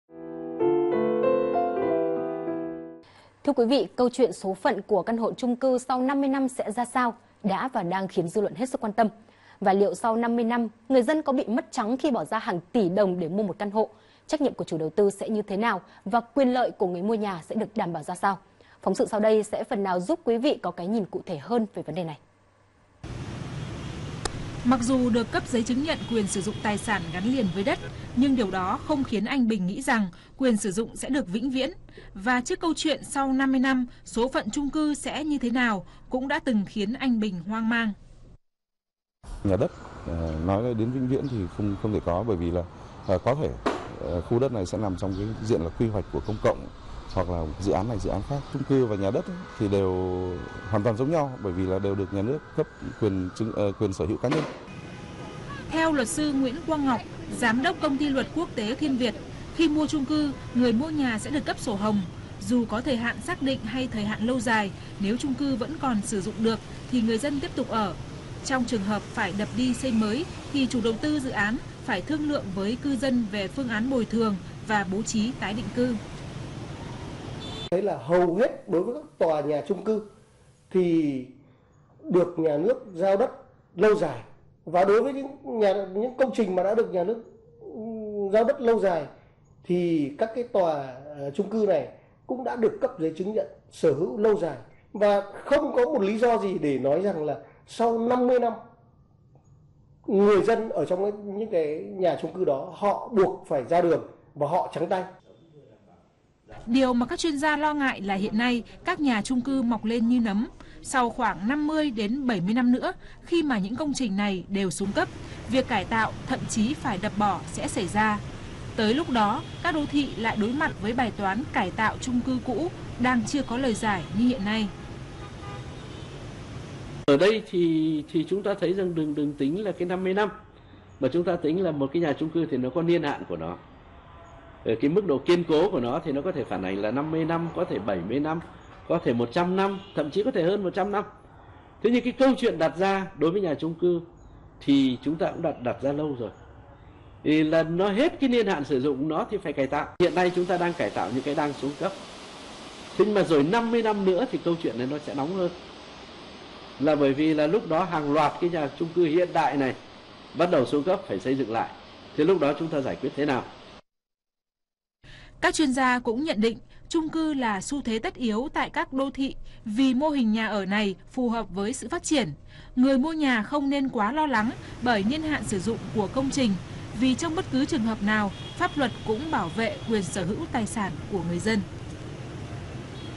Mua chung cư bây giờ, mất trắng sau 50 năm? | VTC
VTC | Thông tin về việc mua nhà chung cư 50 năm sẽ mất trắng gây hoang mang cho nhiều người sở hữu loại hình nhà ở này. Tuy nhiên theo pháp luật quy định thì điều này rất khó xảy ra.